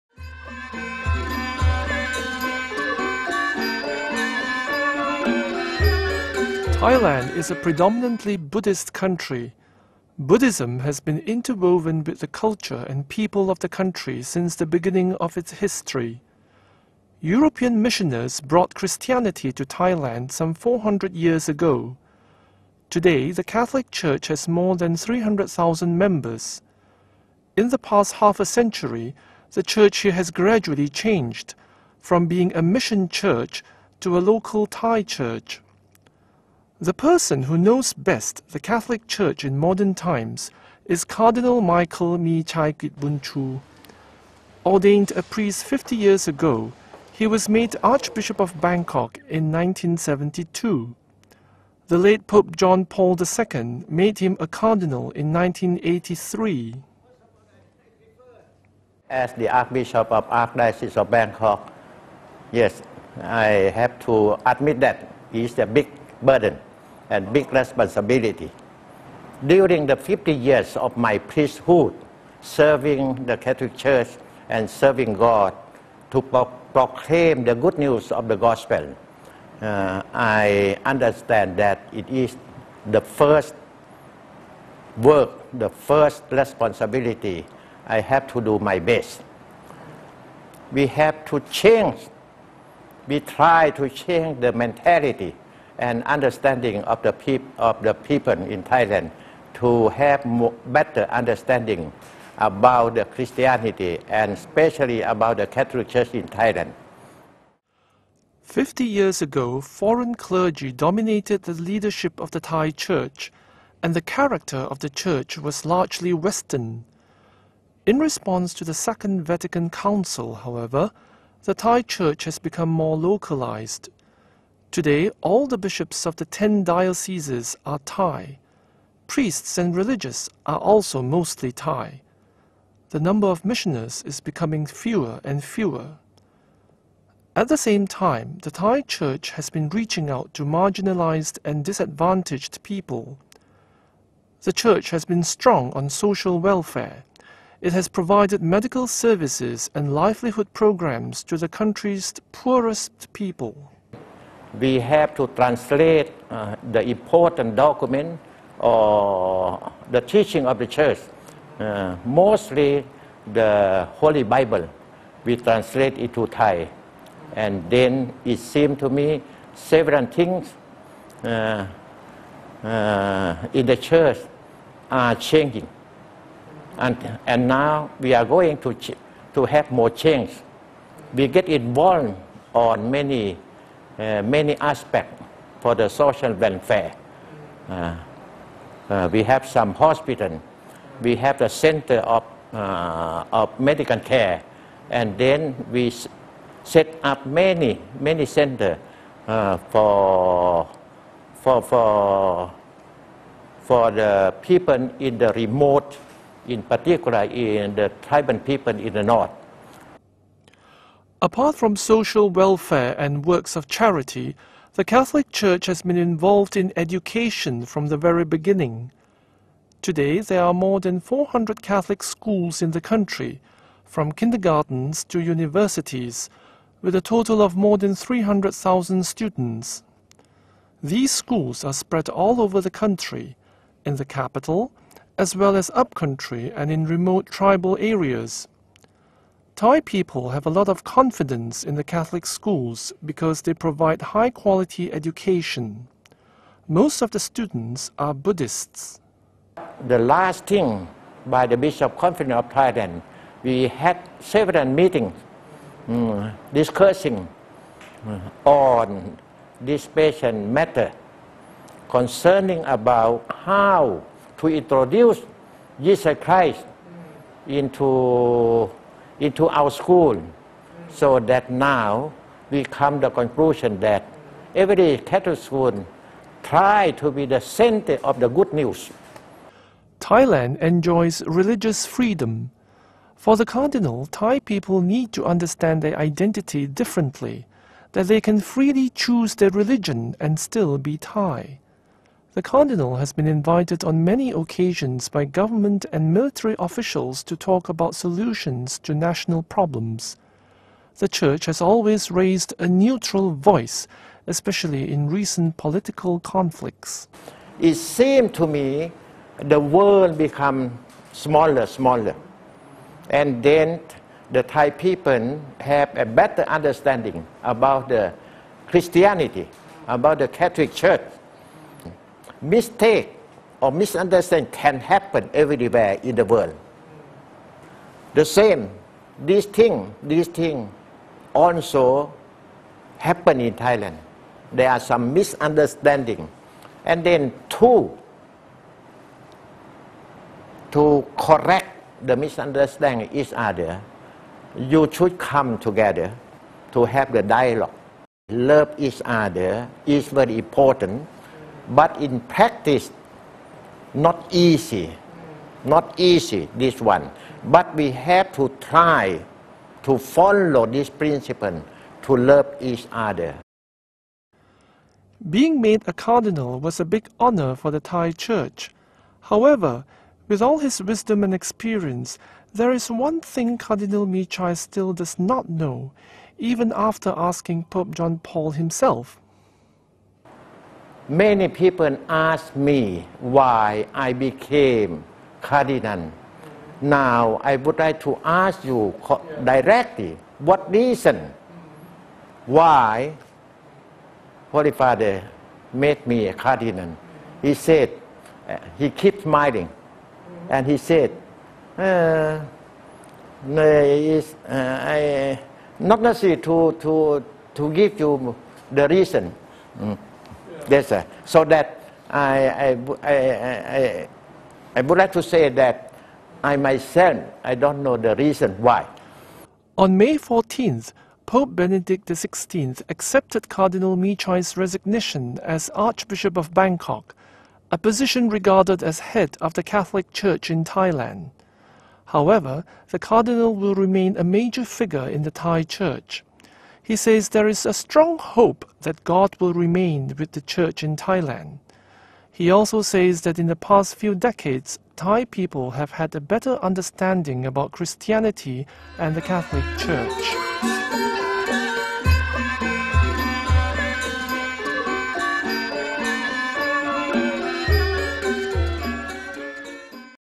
Changing Face of Thai Church
Cardinal Michael Michai Kitbunchu of Bangkok talks about the Catholic Church in Thailand